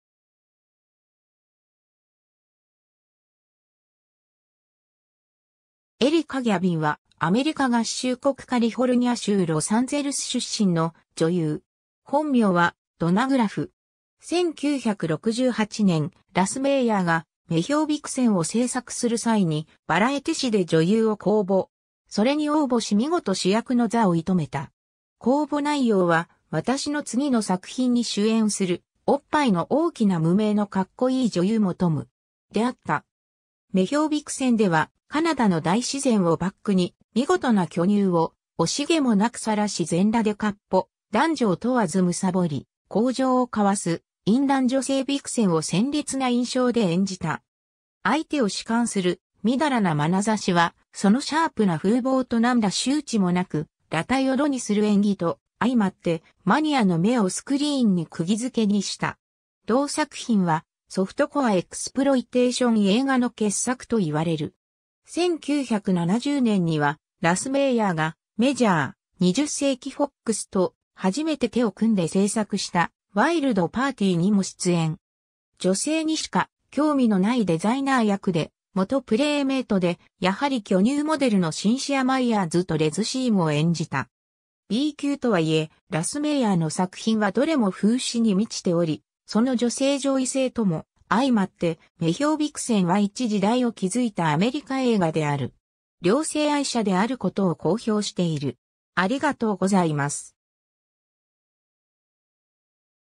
エリカ・ギャビン
エリカ・ギャビン（Erica Gavin、1947年7月22日 - ）は、アメリカ合衆国カリフォルニア州ロサンゼルス出身の女優。
本名はドナ・グラフ（Donna Graff）。
 1968年、ラス・メイヤーが『女豹ビクセン』を制作する際にバラエティ紙で女優を公募。
それに応募し見事主役の座を射止めた。
公募内容は「私の次の作品に主演するオッパイの大きな無名のカッコいい女優求む。」であった。
 『女豹ビクセン』ではカナダの大自然をバックに見事な巨乳を惜しげもなくさらし全裸で闊歩、男女を問わず貪り交情を交わす淫乱女性ビクセンを鮮烈な印象で演じた。
相手を視姦する淫らな眼差しは、そのシャープな風貌と何ら羞恥もなく裸体を露にする演技と相まってマニアの目をスクリーンに釘付けにした。
同作品はソフトコア・エクスプロイテーション映画の傑作と言われる。
 1970年にはラス・メイヤーがメジャー(20世紀フォックス）とはじめて手を組んで制作した『ワイルド・パーティー』にも出演。
女性にしか興味のないデザイナー役で、元プレイメイトでやはり巨乳モデルのシンシア・マイヤーズとレズシーンを演じた。
 B級とは言え、ラス・メイヤーの作品はどれも風刺に満ちており、その女性上位性とも相まって『女豹ビクセン』は一時代を築いたアメリカ映画である。
 両性愛者であることを公表している。